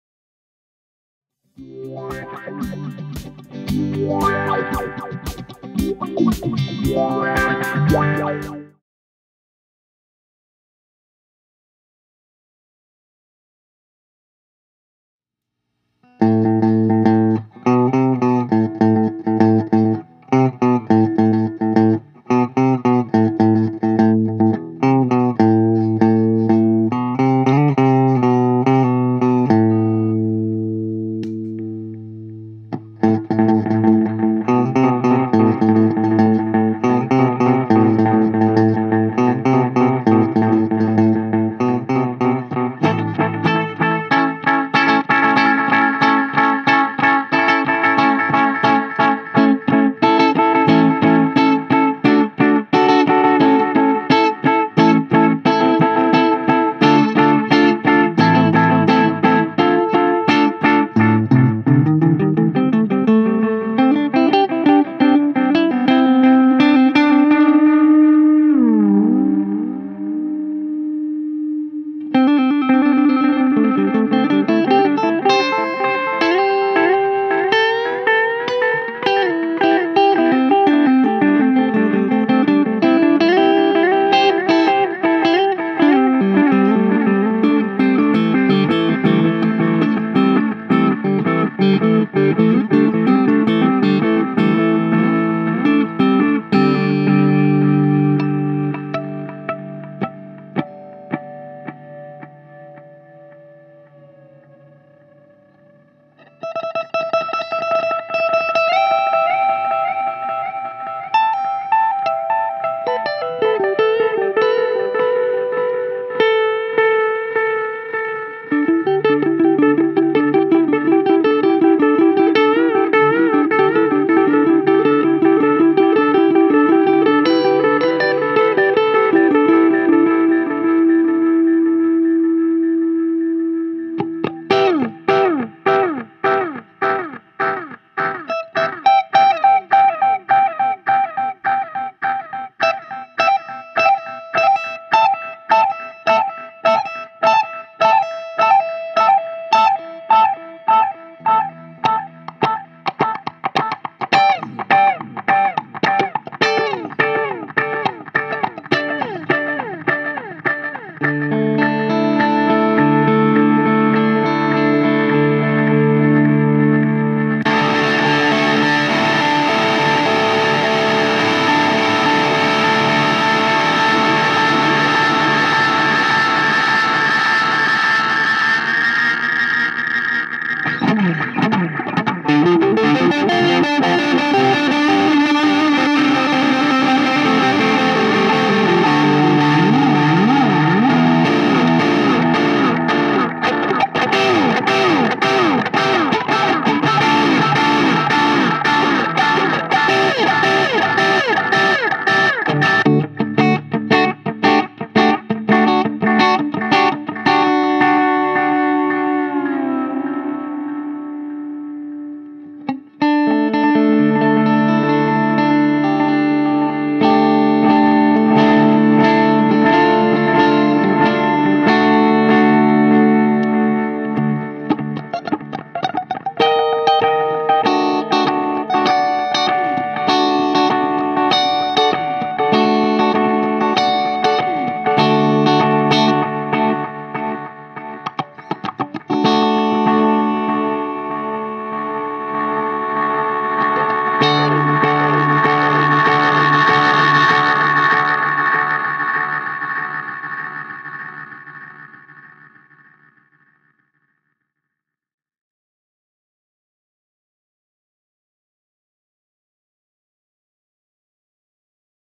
Caline CP-41 Ghost Rain Reverb Delay
This is a demo video of the CP-41 Ghost Rain Reverb Delay effects pedal from Caline performed and produced by Pedal of the Day. for more effects pedal reviews and demos, and be sure to check out more of the videos on our YouTube channel as well. Cheers!

PHRED Instruments Dockstar Spalted Maple loaded with Porter Pickups Smooth/Classic Split-Coil Humbucker pickups

Spectraflex Cables